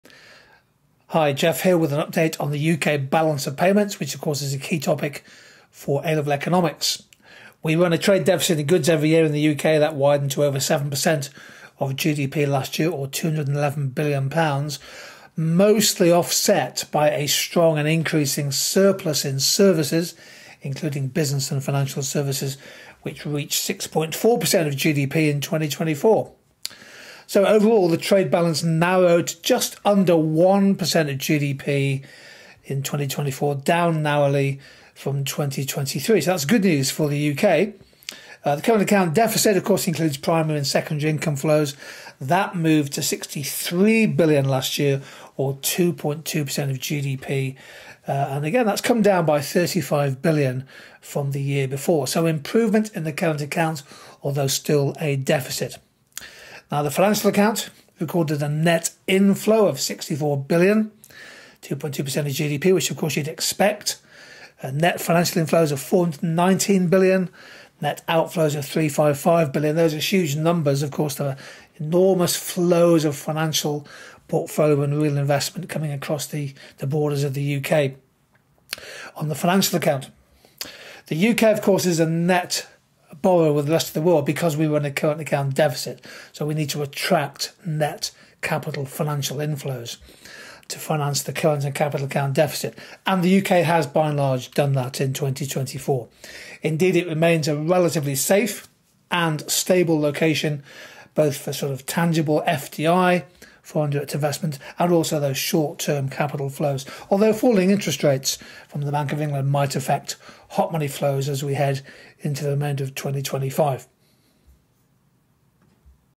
The UK's Balance of Payments | Latest Data Breakdown
Today we’re breaking down the UK Balance of Payments — what it is, why it matters, and the latest data from 2024 you need to know for your exams and economic updates.

The UK is still a net borrower from the rest of the world — it needs those financial inflows to cover its current account deficit.

But the UK remains a safe and stable destination for investment, which helps keep the balance steady.